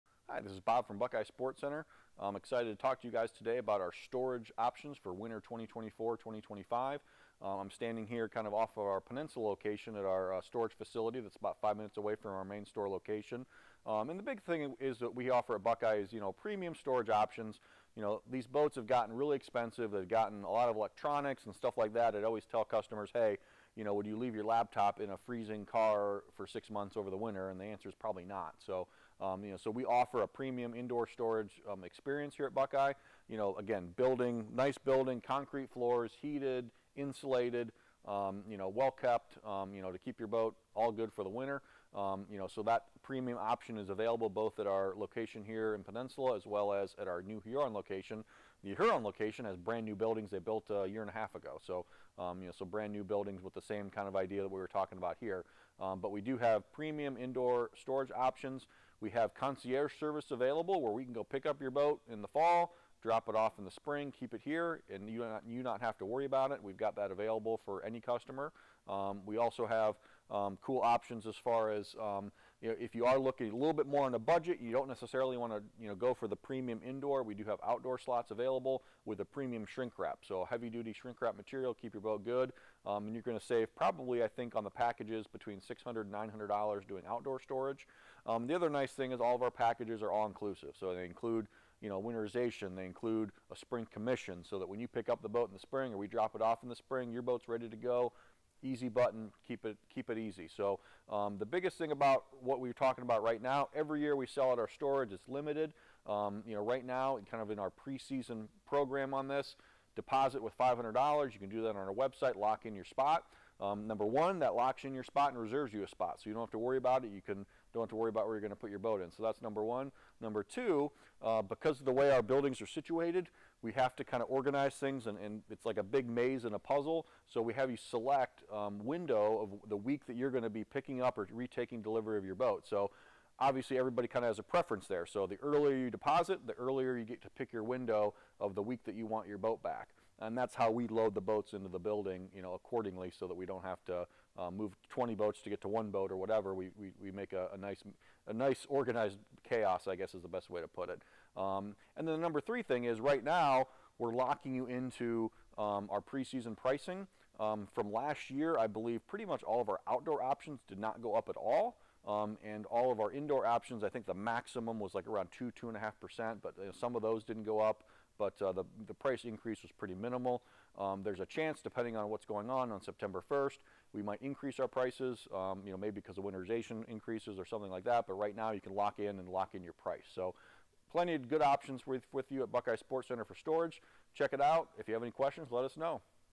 Winter Storage 2024/2025 at Buckeye Sports Center
Winter Storage 2024/25! Reserve today!

Don't miss out and deposit today! Both locations features a state of the art, nice heated building with concrete floors. Buildings in Huron (pictured above) are brand new! None of our boats are stored in barn/dirt floor style buidlings! We also offer lower cost outdoor secure storage with shrink wrap! All our our storage options include full and complete package pricing including winterize, spring commission & more.

Below are some example prices for different types of boats for your reference. There are other packages available as well.